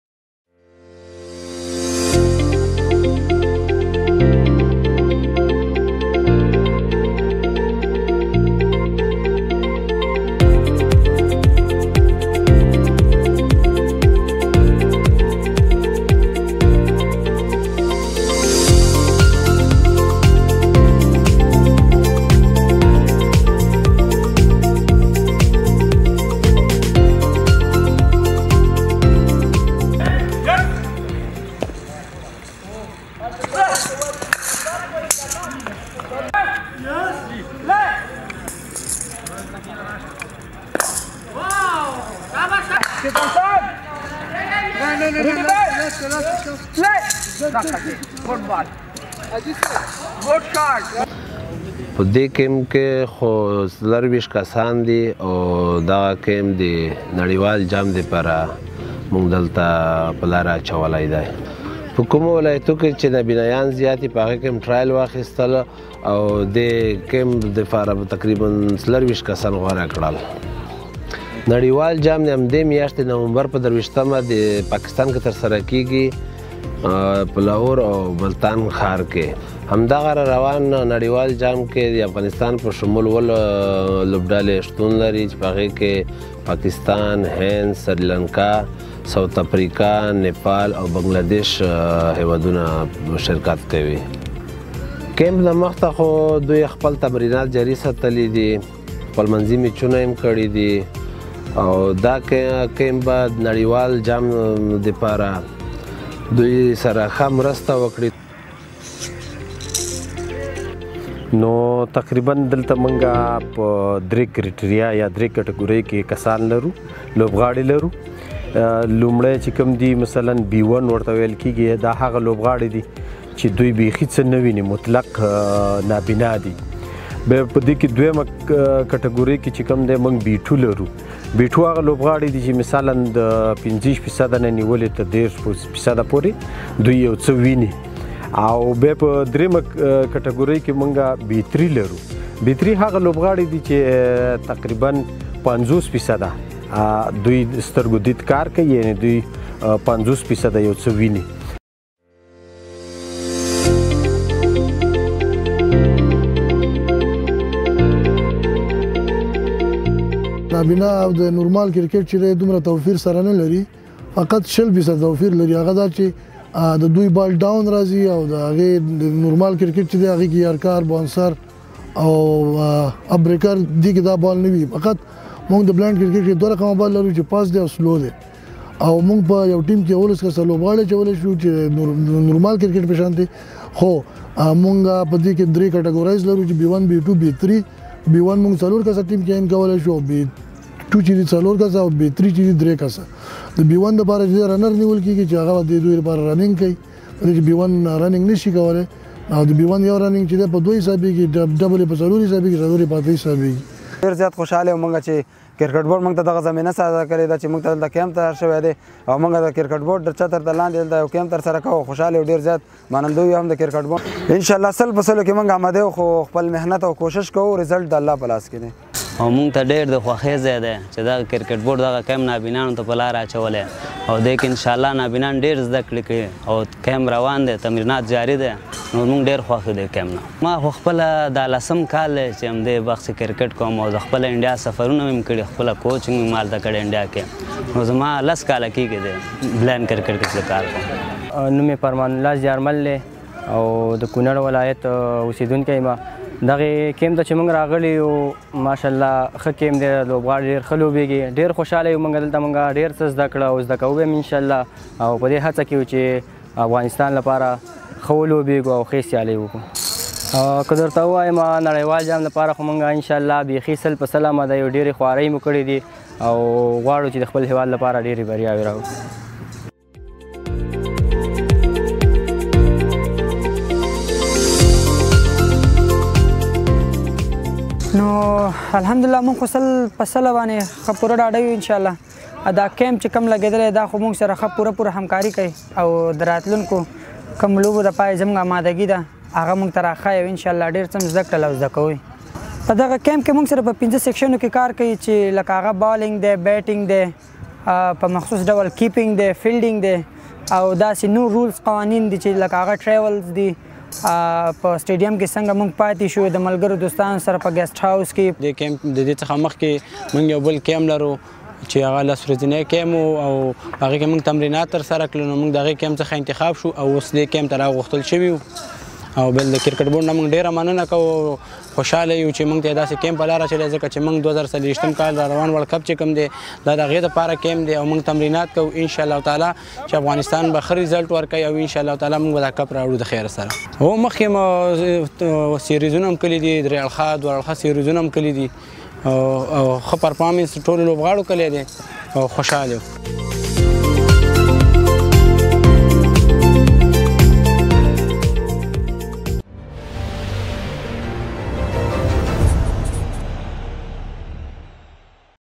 Blinds National Team's World Cup Preparations Underway in Nangarhar Province | ACB
Blinds National Team's World Cup Preparations Underway in Nangarhar Province

Afghanistan's Blind National Team is currently engaged in a training and preparation camp in Nangarhar Province ahead of the 2024 Blind Cricket World Cup, which is scheduled to begin in Pakistan later this month.

The camp includes players from the B1, B2, and B3 visual categories, which are selected through open trials held across all five regions of the country.

The camp will run through November 13, after which the team will travel to Peshawar for two practice matches before embarking on their official World Cup journey in Multan and Lahore.
Welcome to the Official YouTube Channel of the Afghanistan Cricket Board - the sole Governing Body for cricket in Afghanistan. 🇦🇫🏏